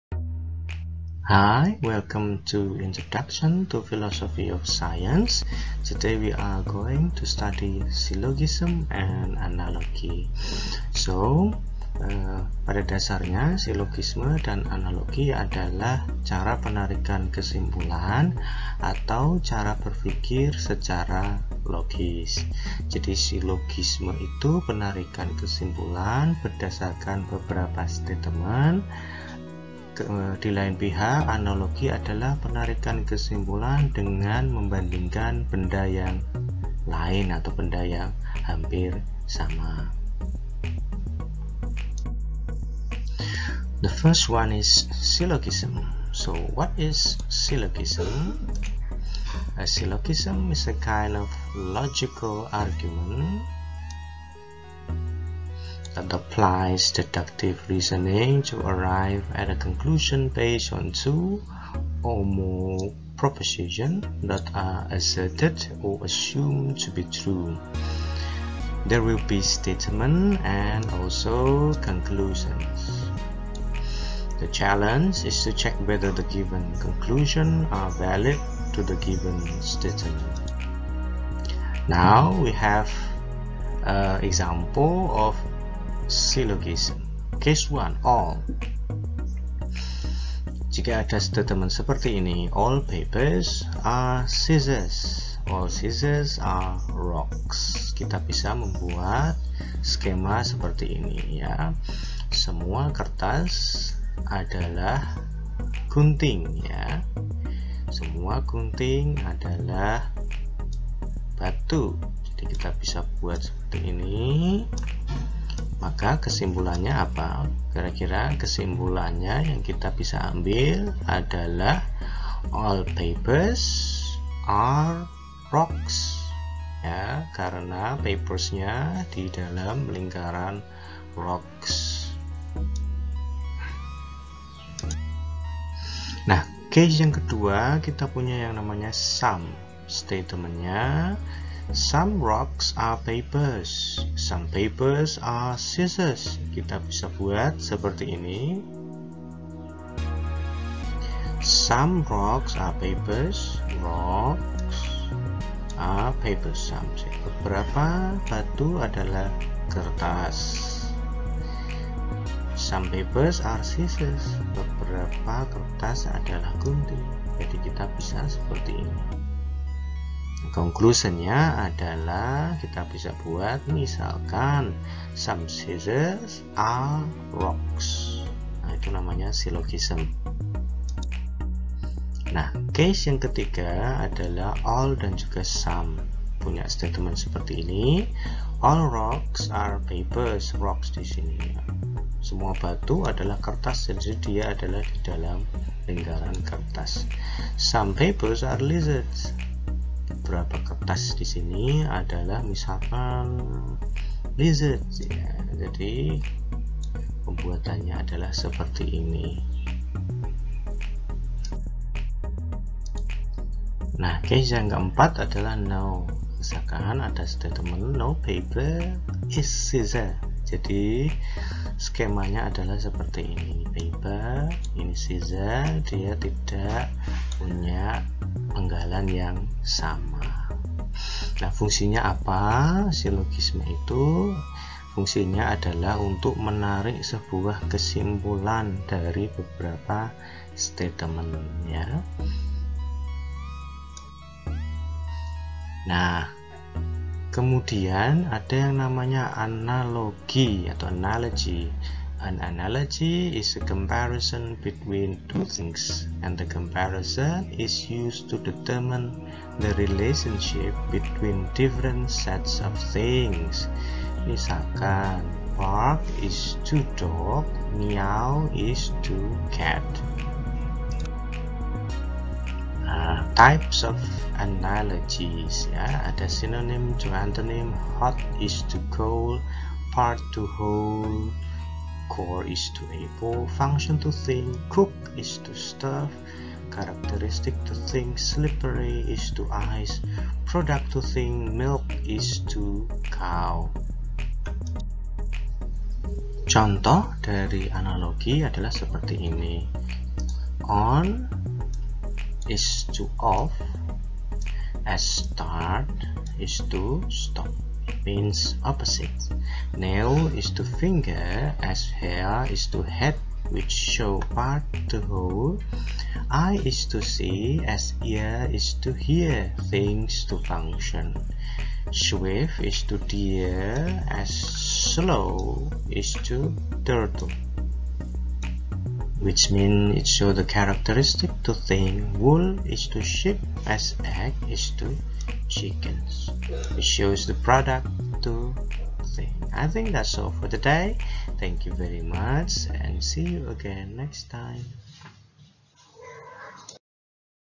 Syllogism and Analogy
Cara penarikan simpulan secara logis yakni menggunakan silogisme dan analogi